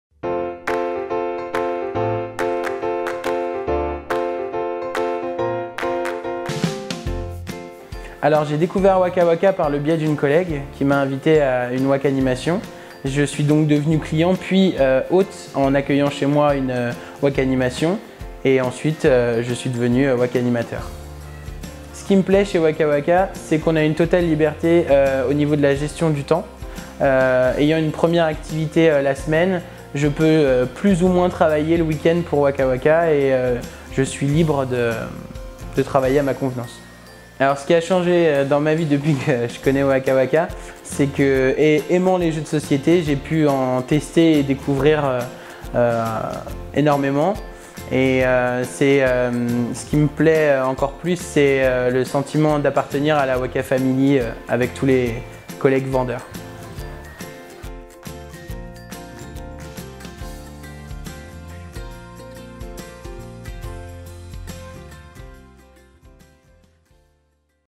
Cédric, Conseiller Oika Oika
❝J'ai pu découvrir énormément de jeux en travaillant chez OIKA OIKA !❞
Comme Cédric, vous êtes fan de jeux de société ?